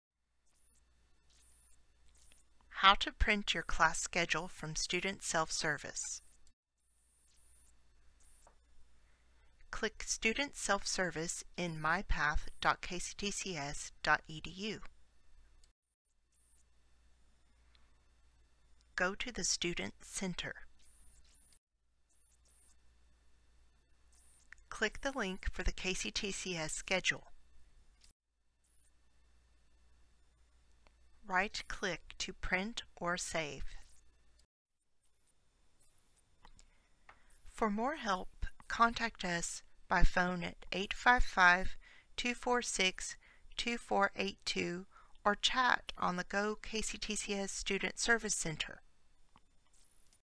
How to Print Your Schedule from Student Self Service
A short introduction to accessing Student Self-Service from MyPath.kctcs.edu and printing a class schedule.